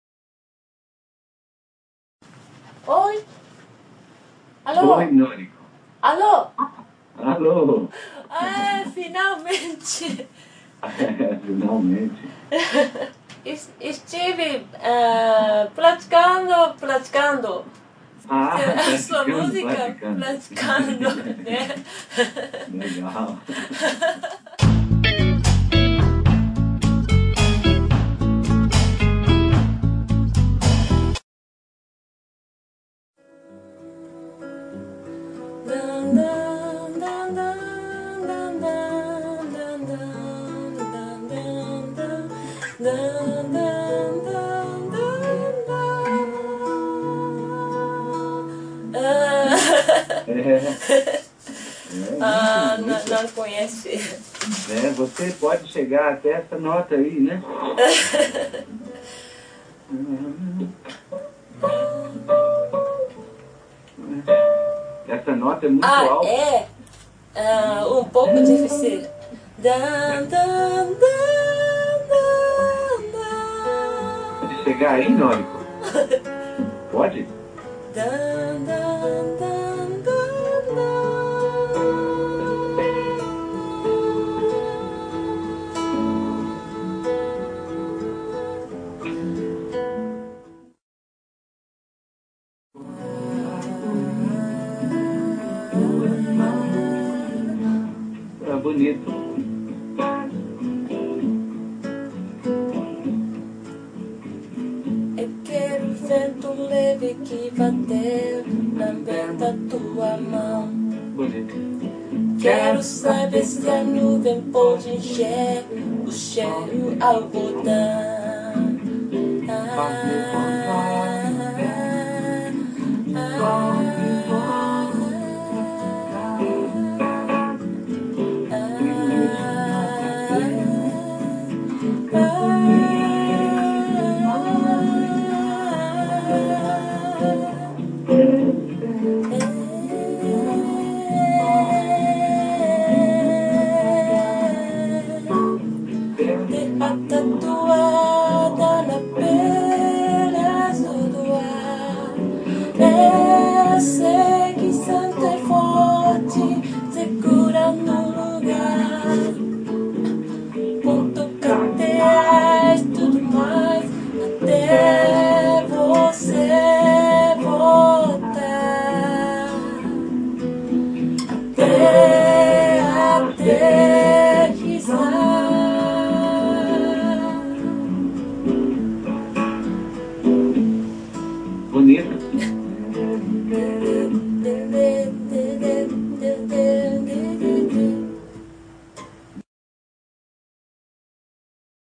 NORIKO YAMAMOTO & LEO MINAX (Noriko meets Leo on the phone)
The first time they talk to each other.